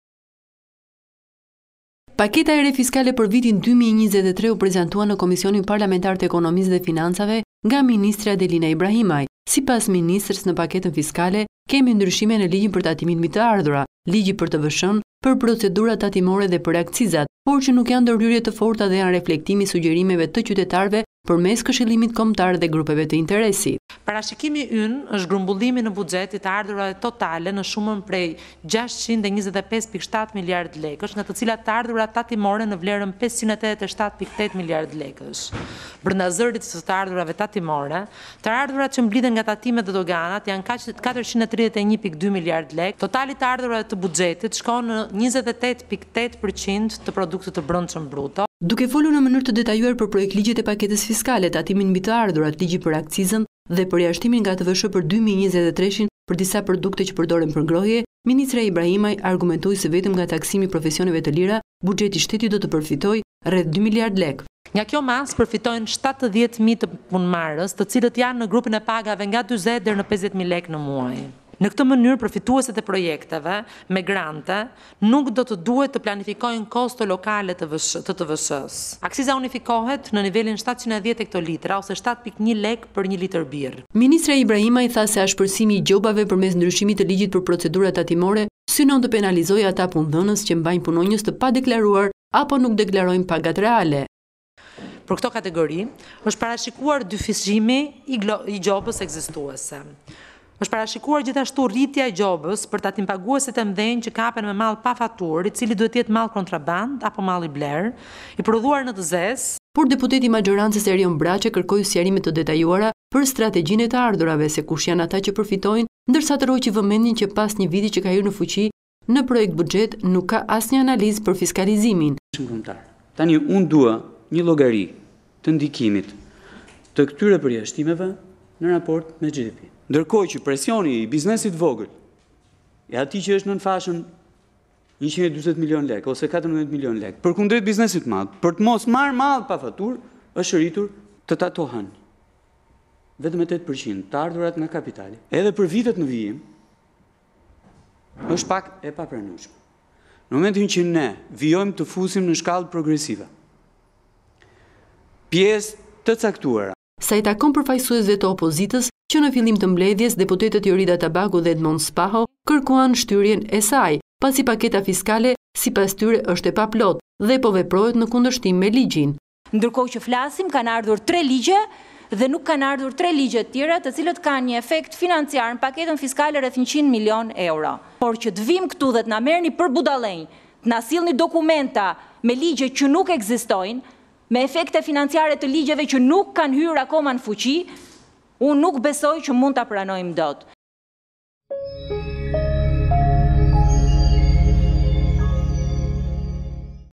Ibrahimaj te 'Ekonomia':Buxheti 2023 do përfitojë rreth 2 mld lekë nga taksimi profesioneve të lira
reportTV lajmeshqip lajmePaketa e re fiskale për vitin 2023 u prezantua në Komisionin e Ekonomisë dhe Financave nga ministrja Delina Ibrahimaj. Sipas ministres, në paketën fiskale kemi ndryshime në ligjin për tatimin mbi të ardhurat, ligji për TVSH, për procedurat tatimore dhe për akcizat, por që nuk janë ndërhyrje të forta dhe janë reflektim i sugjerimeve të qytetarëve përmes Këshillimit Kombëtar dhe grupeve të interesit.